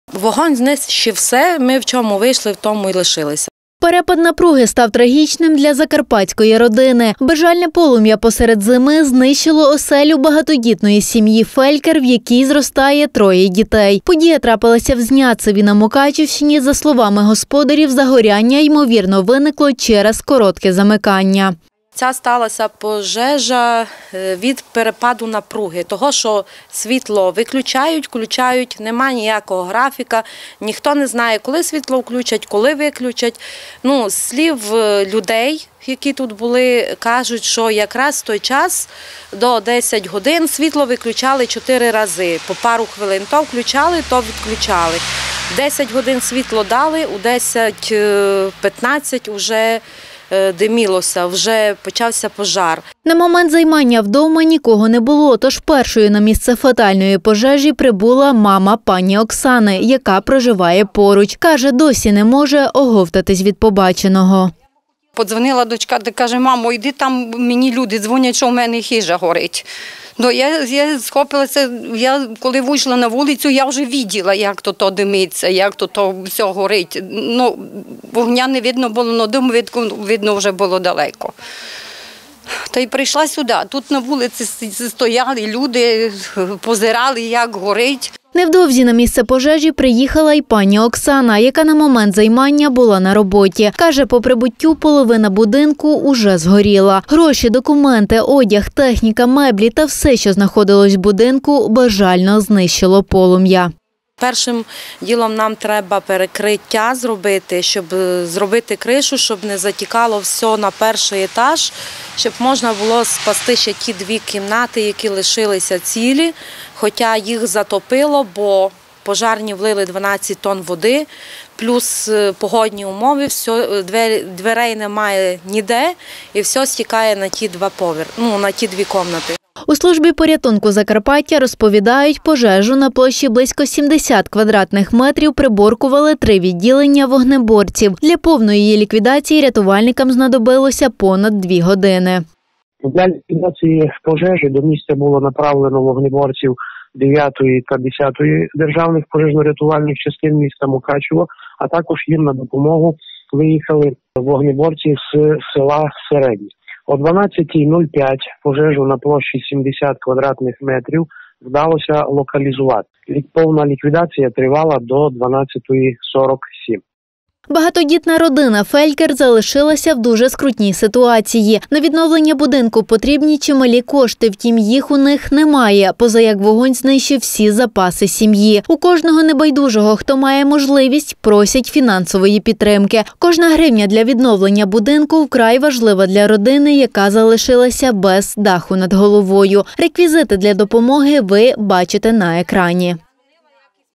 Вогонь знищив весь будинок: родина із Мукачівщини потребує допомоги
Родина Фелькер із Зняцева на Мукачівщині потребує фінансової допомоги. 8 грудня в їх будинку трапилося масштабне займання. Нищівне полум’я вщент знищило все до знаходилось у оселі, разом із документами та запасами коштів. Посеред зими багатодітна сім’я, в якій зростає троє діток, залишилася без даху над головою. Просять кожного, хто має можливість долучитися до допомоги. Реквізити для допомоги: ПриватБанк -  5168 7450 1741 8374 (ФЕЛЬКЕР ОКСАНА ІВАНІВНА)